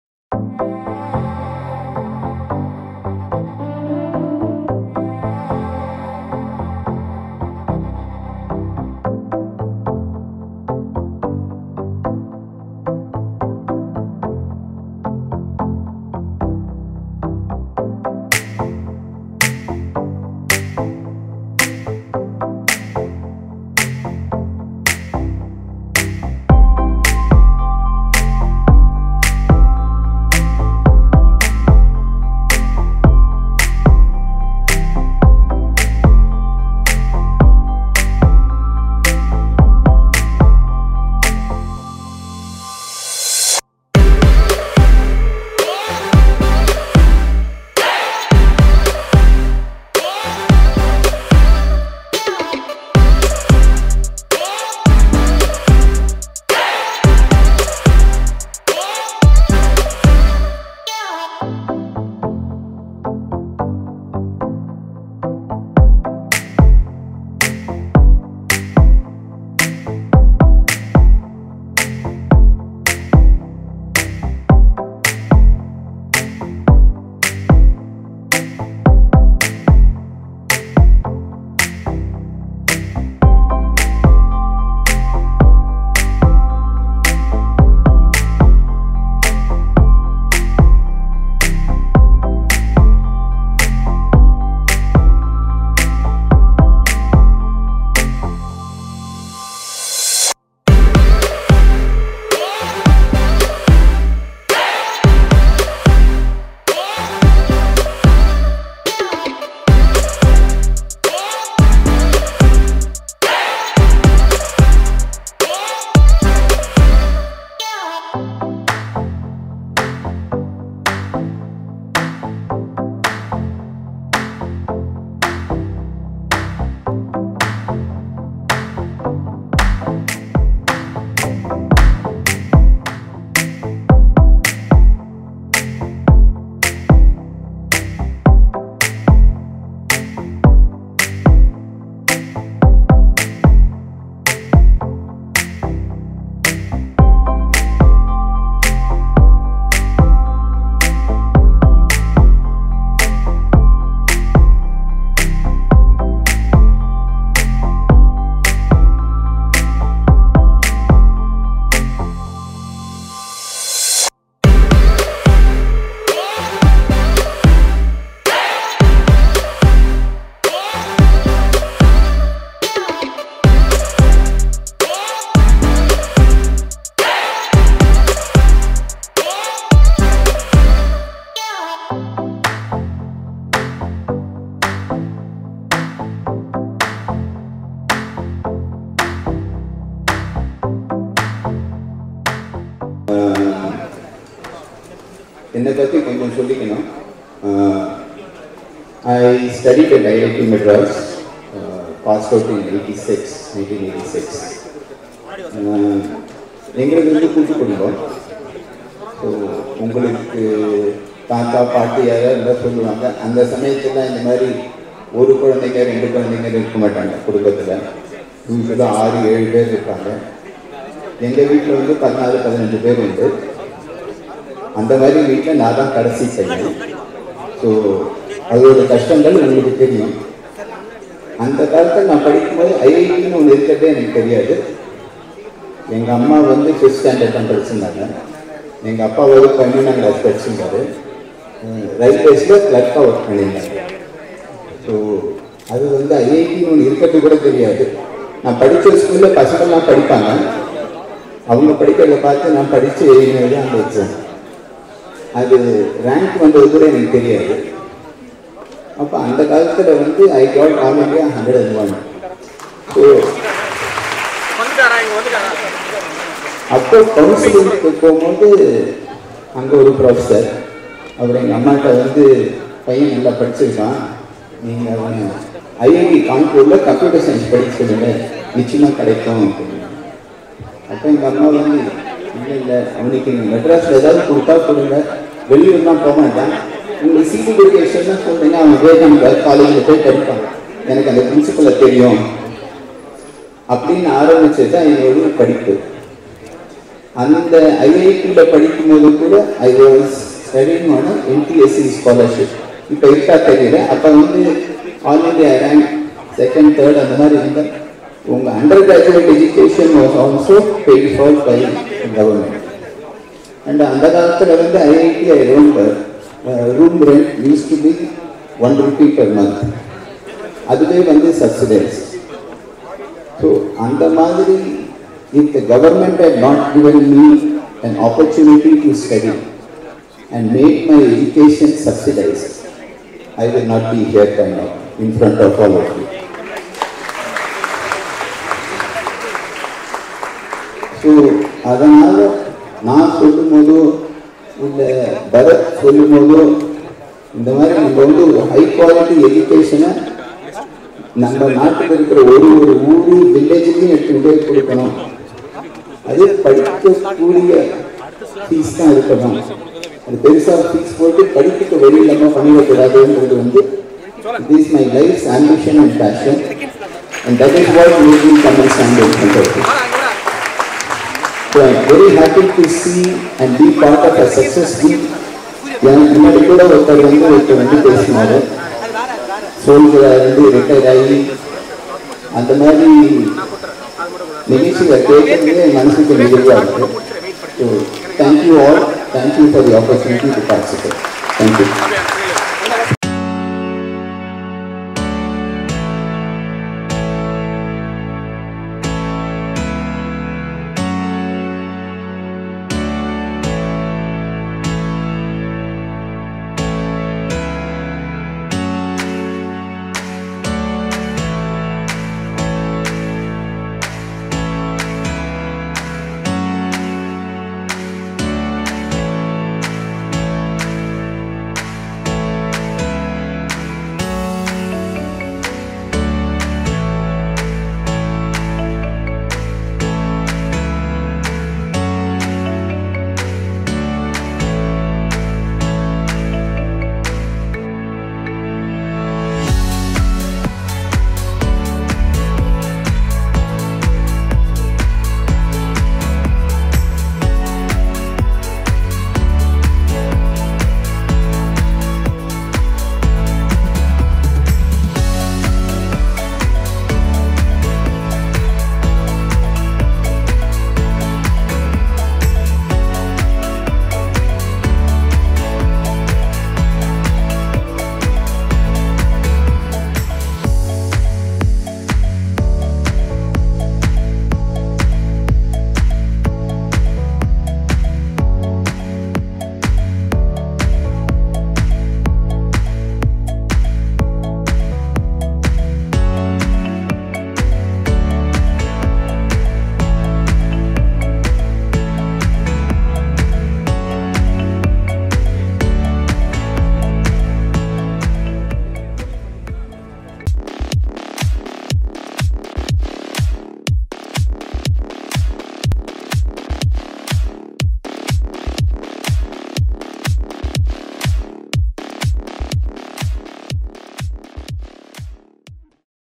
AGS Owner Kalpathi Suresh Motivetional Speech | VERANDA RACE - 10th Year Success Meet | AGS Group
10th Year Success Meet and Felicitation to 1000+ Student Achievers of VERANDA RACE